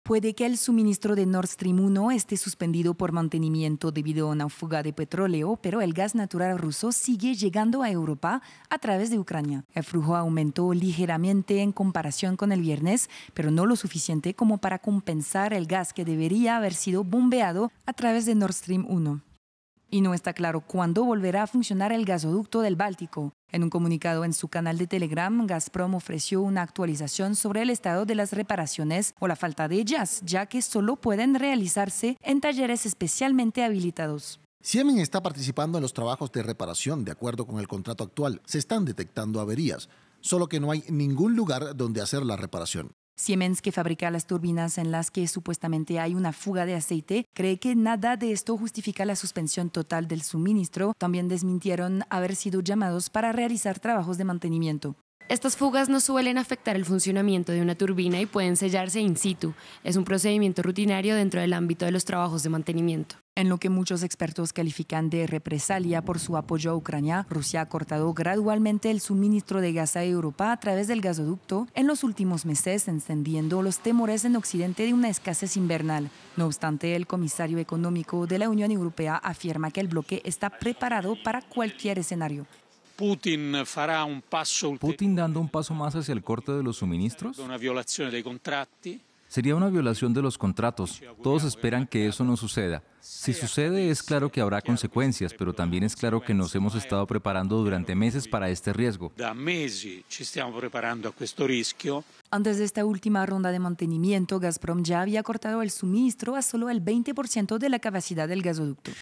Ante el cierre del gasoducto Nord Stream 1, Bruselas dice que no se dejará "intimidar" por Moscú
Los países de la UE se han estado preparando para la posibilidad de que Rusia detenga las entregas por completo, luego de que Gazprom redujera los flujos primero en junio, luego nuevamente en julio y que empezara a hacer cortes de suministro y mantenimientos del gasoducto de manera reiterada e indefinida en los últimos días.